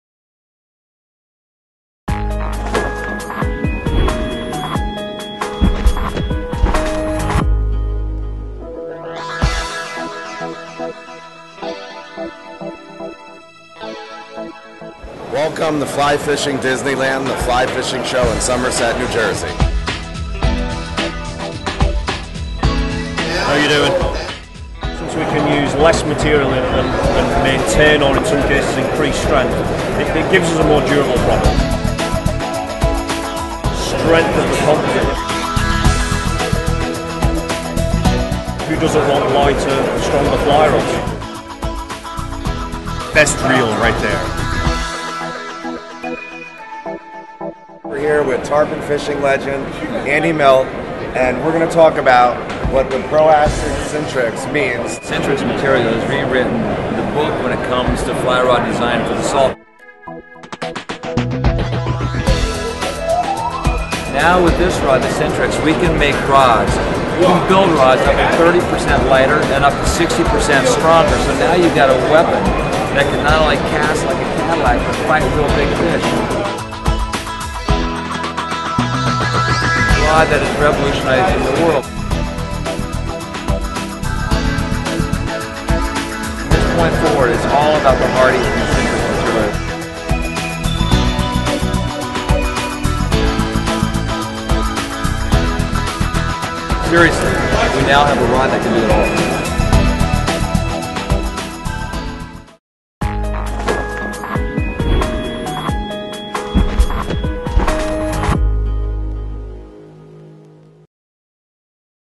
TPO Somerset Fly Show Short - Hardy | Greys Fly Fishing Show Somerset 2011
Quick overview of what was new at Hardy | Greys this year at the Fly Fishing Show in Somerset NJ. Also introducing Hardy's revolutionary Sintrix technology. Featuring Aaron Jasper, Howard Croston, and Andy Mill. Directed, Edited and Produced by Taylor Vavra of Leviathan Productions.